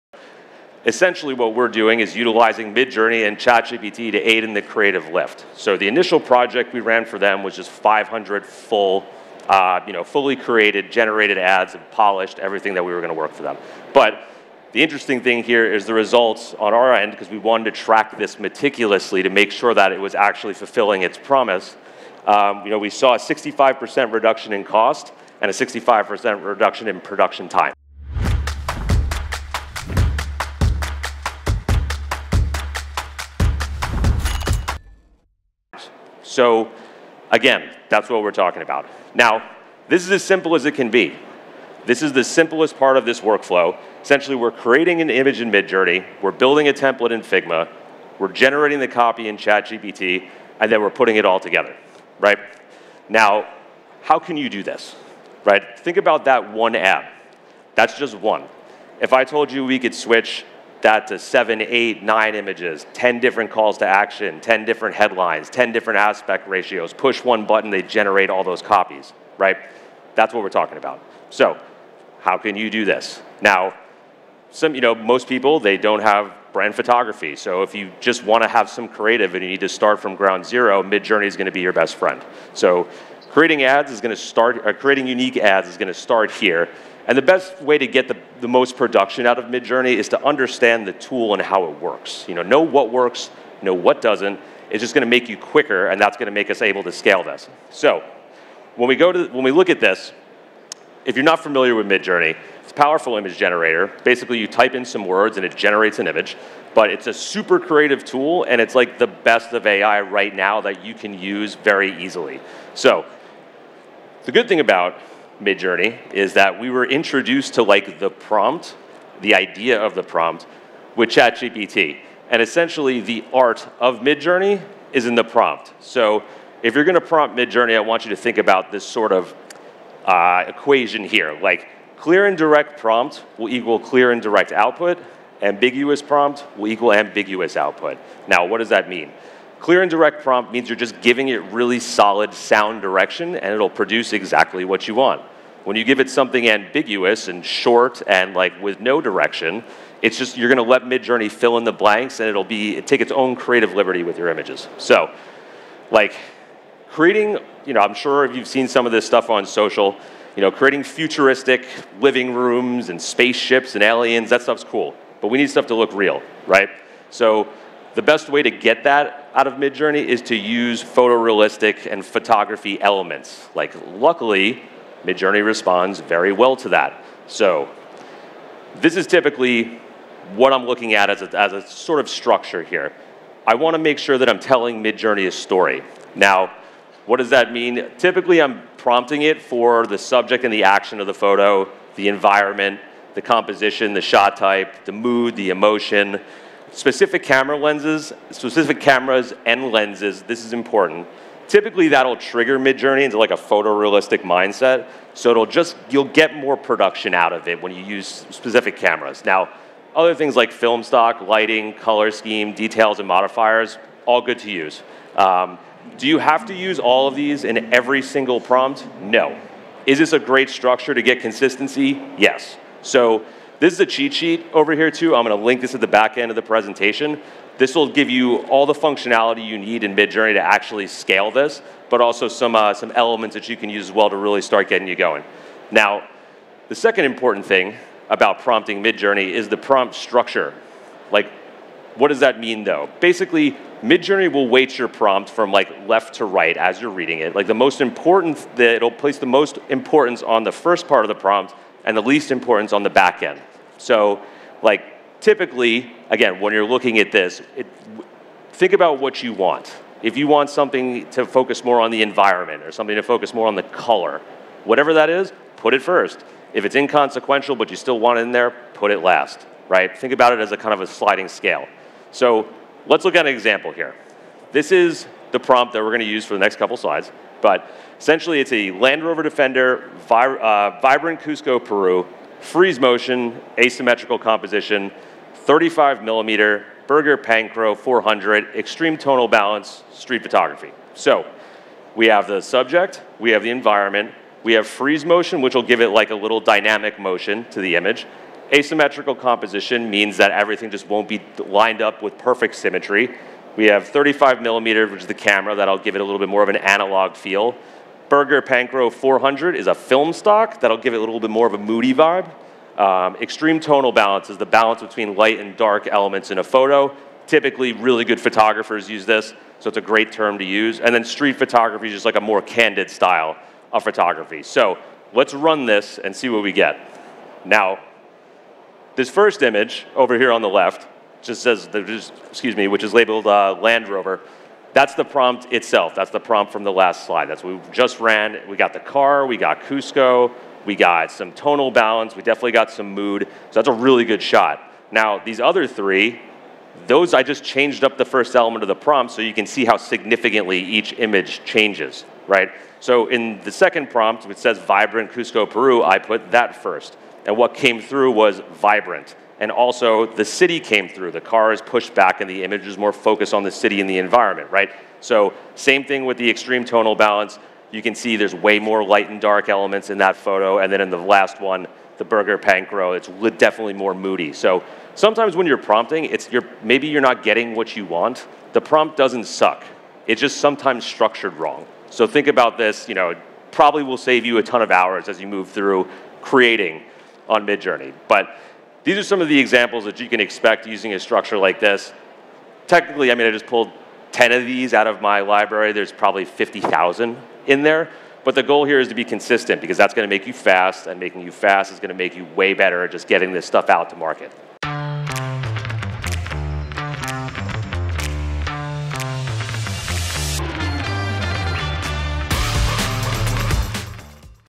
Midjourney Prompt Strategies for Photorealistic AI-Driven Ad Creatives
Harnessing Midjourney & ChatGPT for Revolutionary AI Ad Creatives

Rory Flynn, co-founder of Systematiq.ai, has revolutionized business and marketing operations by integrating AI tools, enhancing productivity, streamlining finances, and transforming client interactions. Previously, as the Head of Client Acquisition at Commerce12, he played a pivotal role in driving over $100M in sales, significantly boosting client acquisition through strategic email and SMS marketing.

In this insightful video, Rory delves into the transformative use of Midjourney and ChatGPT in revolutionizing ad creative production. Discover how a simple yet powerful prompt strategy can drastically reduce costs and production times by 65%. Rory breaks down the art of crafting precise prompts, ensuring Midjourney generates photorealistic, high-impact visuals for your ads.

Learn to navigate Midjourney's potential for creating unique, compelling ad creatives from scratch. Rory guides you through the nuances of prompt structure, revealing how specific directions can yield precise outcomes, while ambiguous prompts leave room for Midjourney's creative interpretation. This video is a treasure trove of insights on leveraging AI tools like Midjourney and ChatGPT to enhance your advertising campaigns, with real-world examples demonstrating the power of effective prompt crafting.

By the end of this tutorial, you'll have a deeper understanding of Midjourney's prompt mechanics and how to exploit them for producing eye-catching, photorealistic AI ad creatives that stand out in today's competitive digital landscape.

🚀 Don't miss out on actionable tips and tactics that can revolutionize your marketing game. Buy your tickets early to secure the best deals!